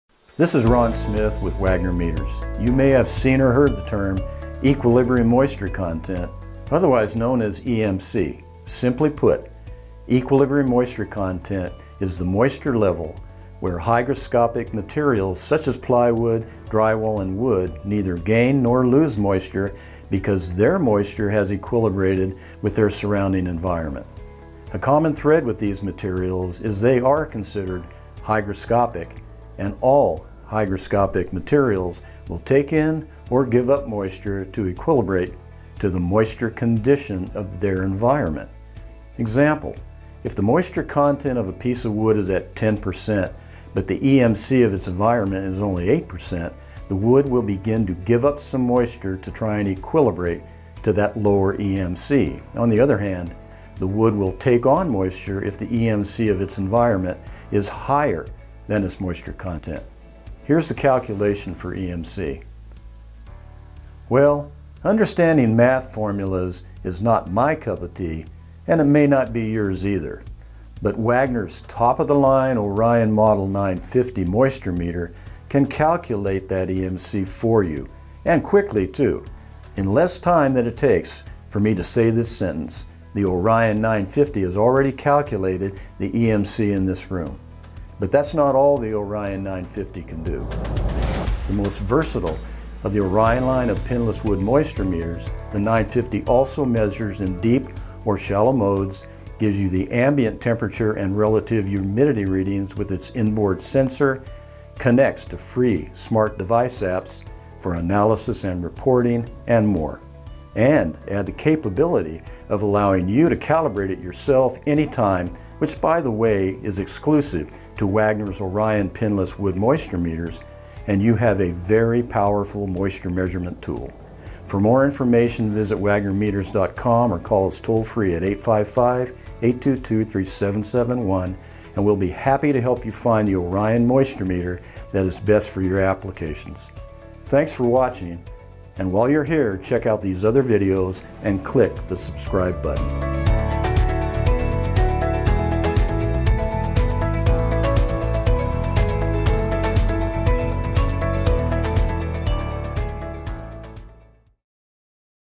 Equilibrium Moisture Content - What is it? [EMC]
You've no doubt heard or seen the term equilibrium moisture content, or "EMC" as it's referred to sometimes. Put simply, EMC is the moisture level where a hygroscopic material neither gains nor loses moisture since it is at equilibrium with the surrounding environment. Ron explains the term as well as introducing the Orion 950 Smart Pinless Wood Moisture Meter as an effective tool to calculate EMC. Determining EMC is a click away.